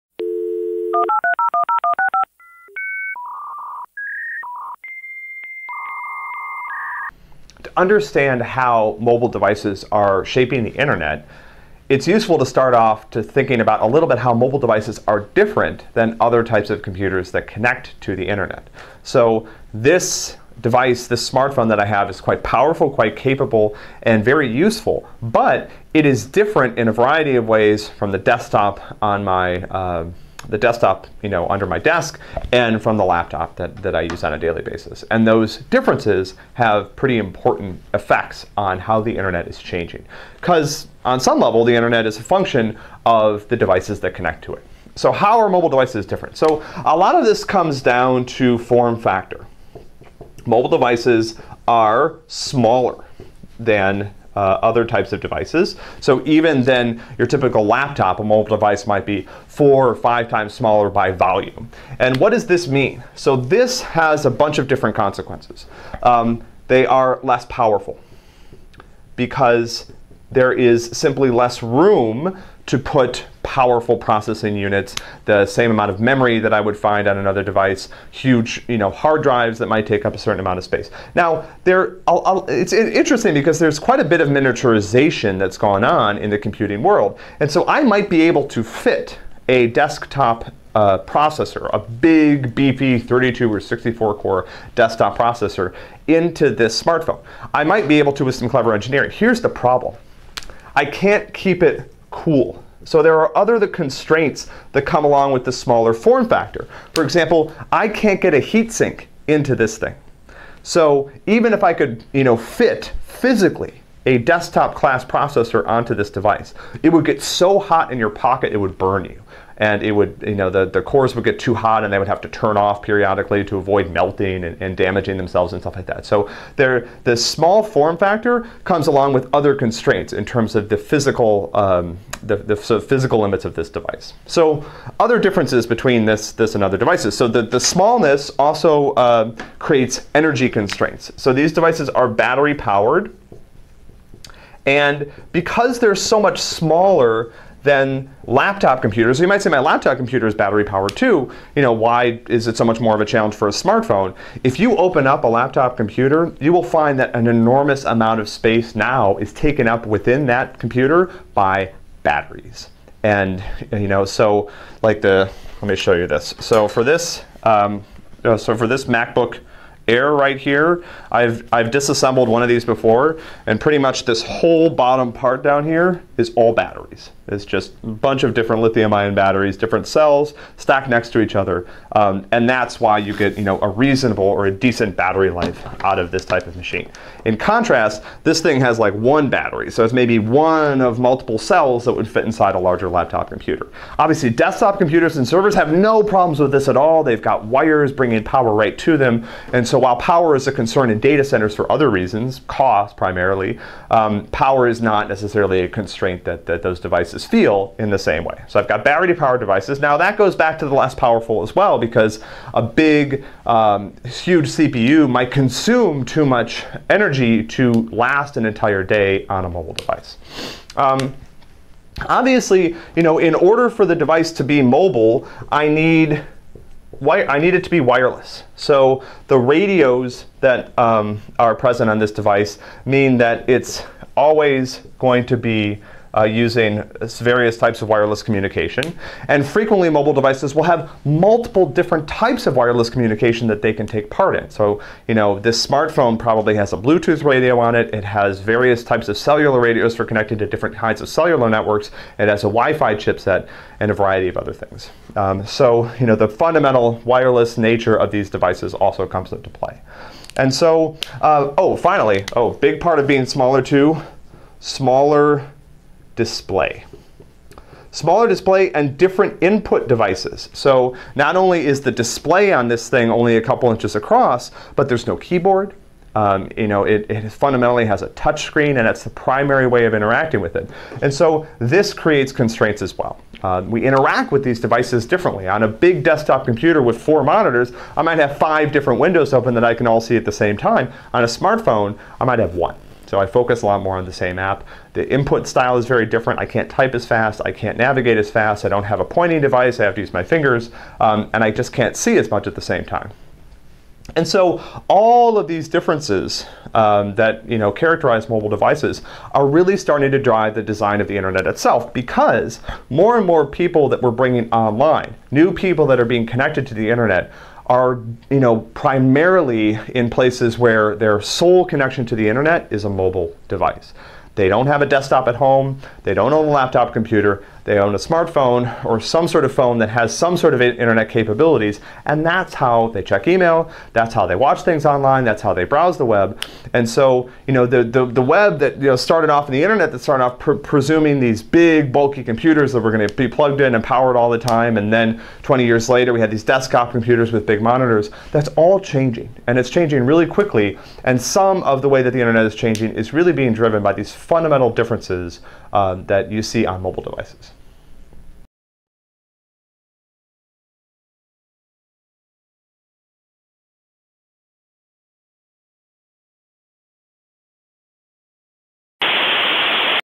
What are some consequences of mobility?
Mobile devices differ from traditional desktop and even laptop computers in many important ways. They are dependent on wireless connections, smaller and less powerful, and battery-powered and so energy-limited. More importantly, mobile devices also experience sudden changes inconditions caused by mobility. Responding to these differences and changes is what makes mobile computing hard.